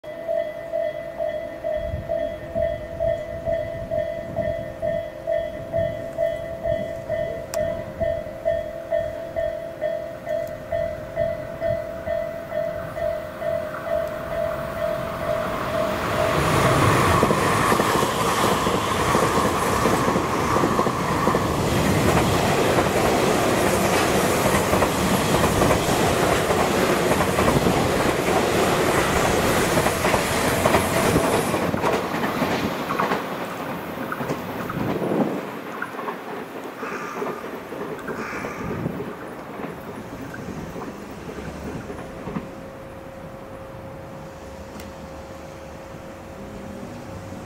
【スペーシアX 連日試運転】東武N100系 スペーシアX N102F 試運転 浅草行 一ノ割～春日部通過【4K】
2023/5/31 東武伊勢崎線(東武スカイツリーライン) 一ノ割～春日部にて

2023年5月29～31日に北春日部→浅草→鬼怒川温泉→下今市→東武日光→浅草→北春日部にてN102Fを使用した試運転が行われました。乗務員訓練と思われます。